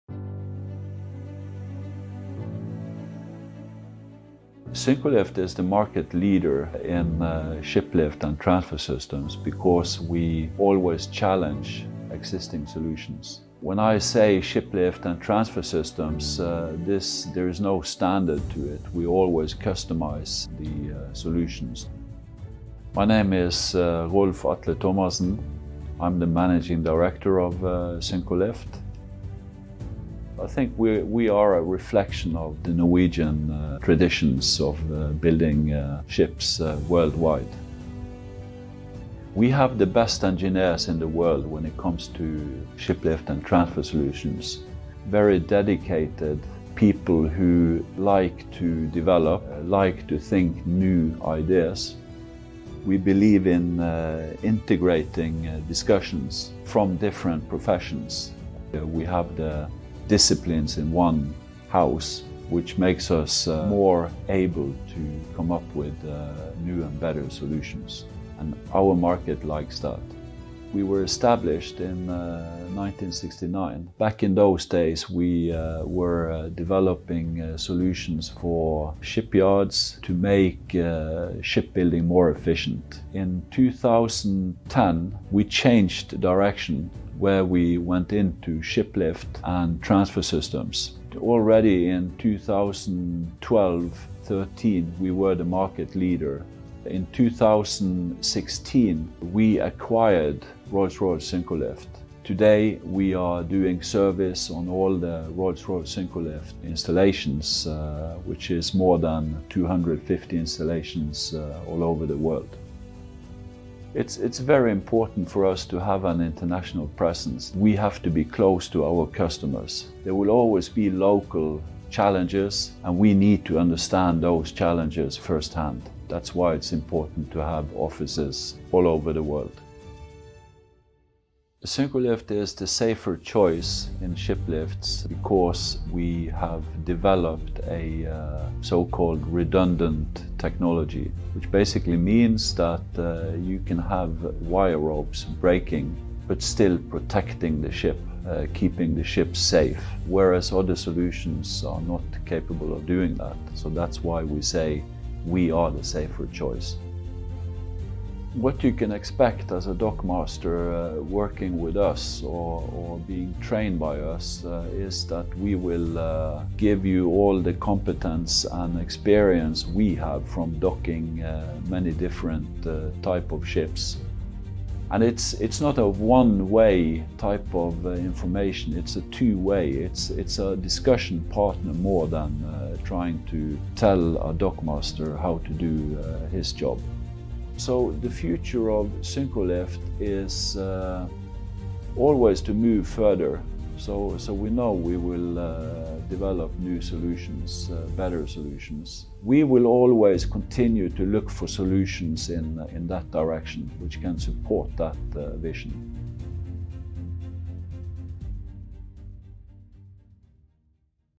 Syncrolift Brand Film
This is a 3 minute video showcasing Syncrolift and how we approach building efficient and safe shiplifts and transfer systems.

Enjoy it and send it to friends and colleagues that are considering upgrading their shipyard. Syncrolift can help!

for more information.